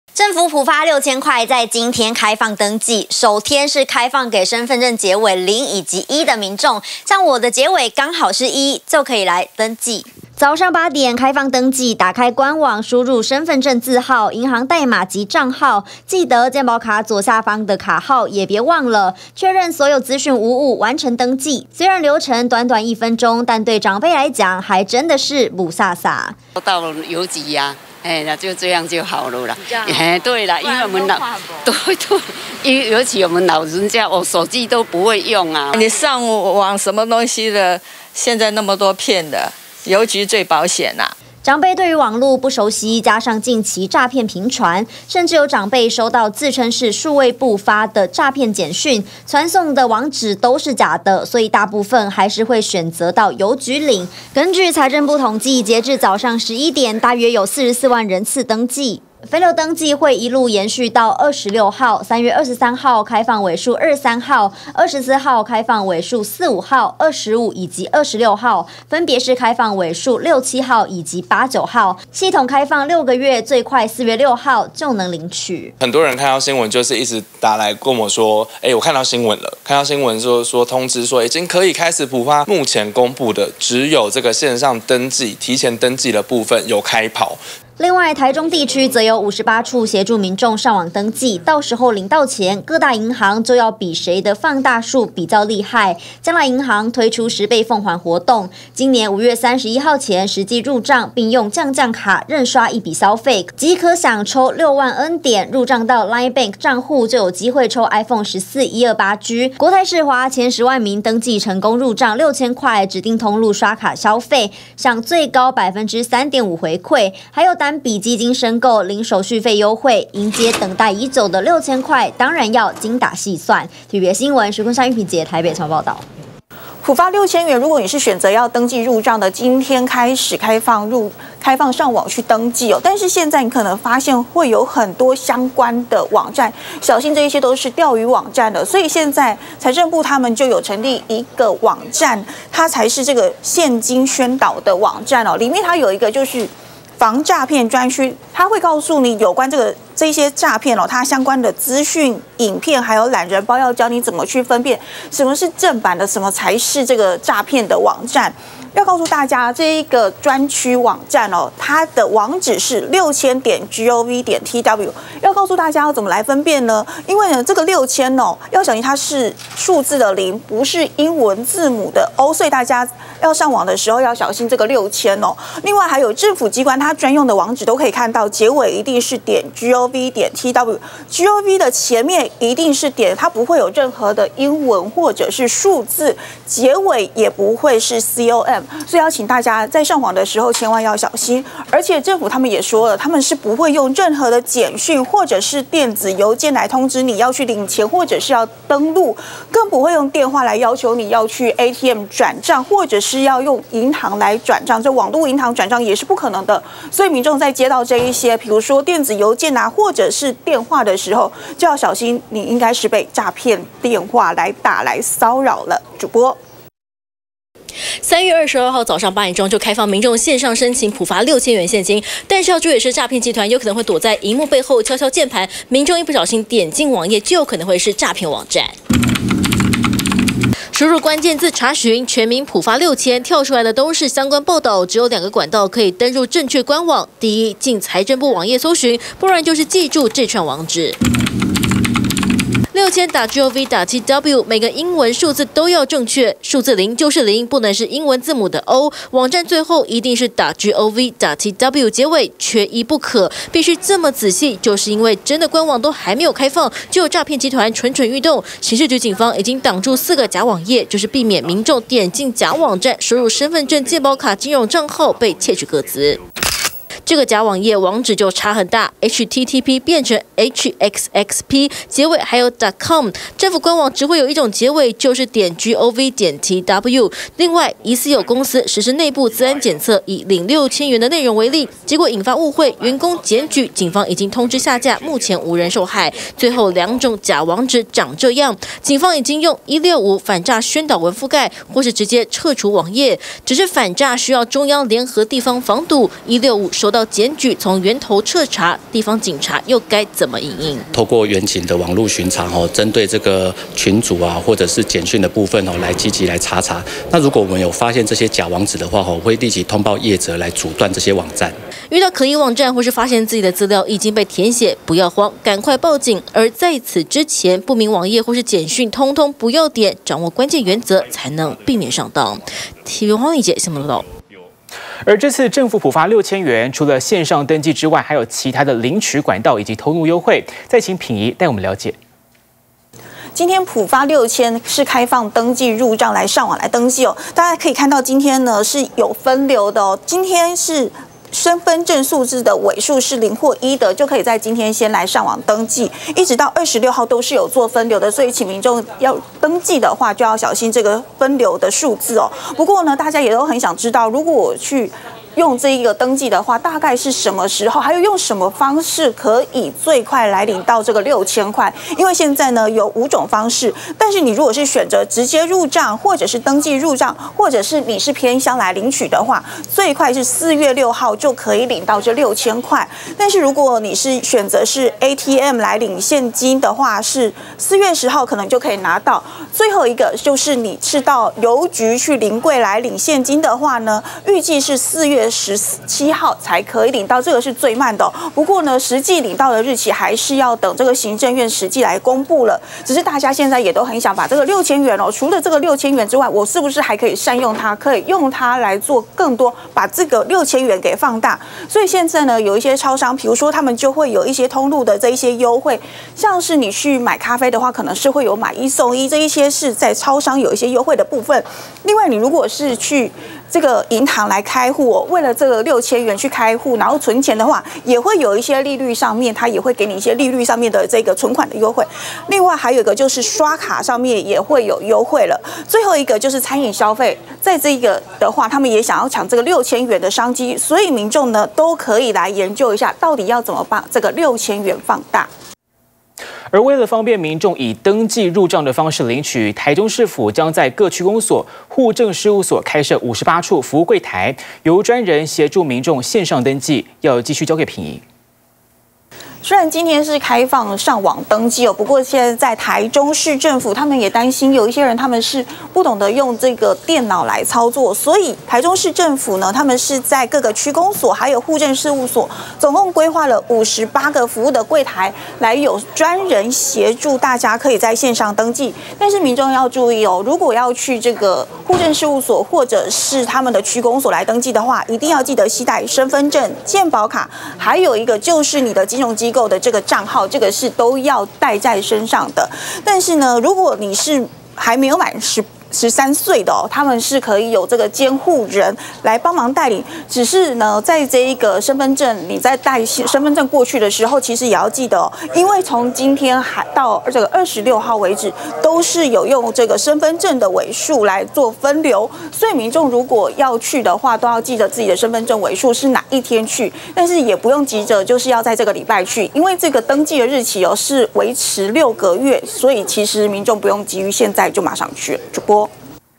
【6000元普發懶人包】6000元登記一次看懂｜TVBS新聞@TVBSNEWS01
政府普發6000元，今天（22號）起開放登記，第一天身分證尾數0以及1的民眾能登記，實際上網走一邊流程，系統順暢的話約1分鐘能完成，而普發現金6000元今天上午才正式開放民眾預先登記，就已經出現詐騙的釣魚網站，為了防止被詐騙財政部有什麼提醒；以及除了線上登記之外，還有哪些領取管道及通路優惠，TVBS帶你了解。